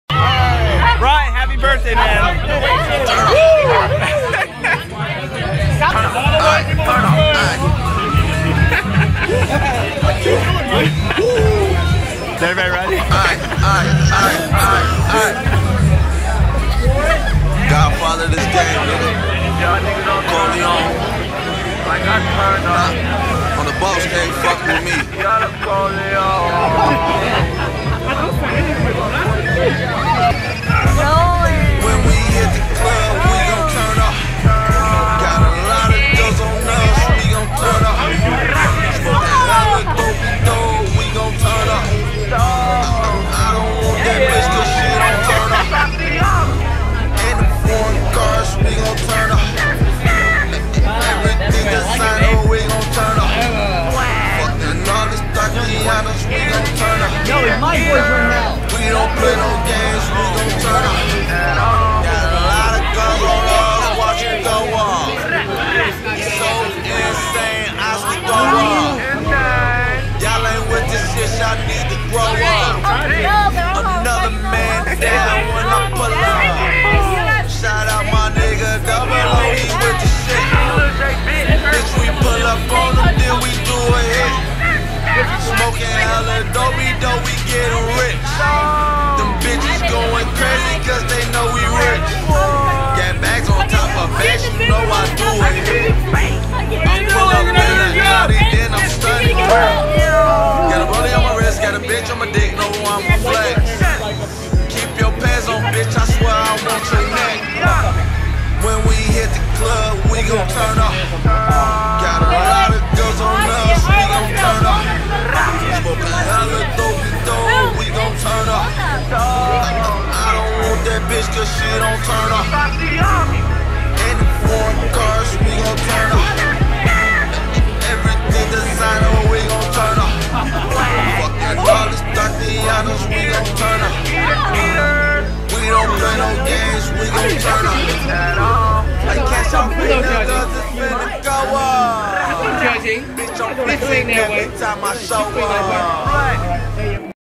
Battle and Brew - Brian's B-Day Bash - 05/16/14
Highlights from Brian's Birthday Celebration at Battle and Brew. This was after we saw Godzilla. Had a hell of a night, good times with all these people. I think this video captures the spirit of the night.